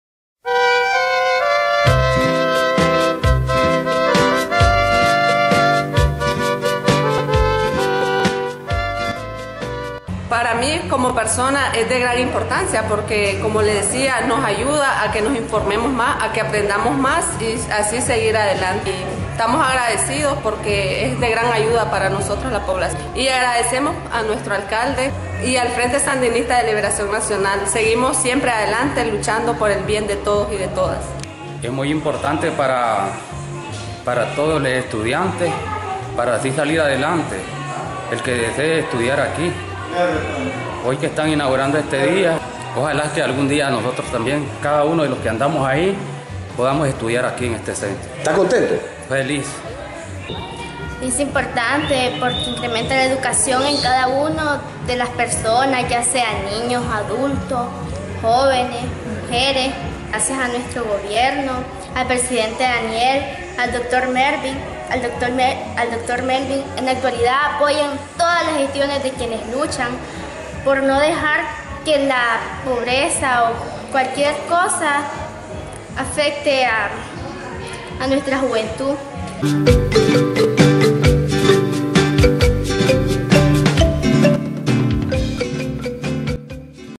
LOS JÓVENES OPINAN SOBRE ESCUELA TECNOLÓGICA.
Jóvenes de ambos sexos, expresan su opinión respecto a la construcción de este Centro Tecnológico en Totogalpa, el cual beneficiara a mas de 200 protagonistas en los cursos de computación gratis que la Alcaldia Municipal y el Gobierno Central brinda a las y los jóvenes, niños y adultos de forma gratuita.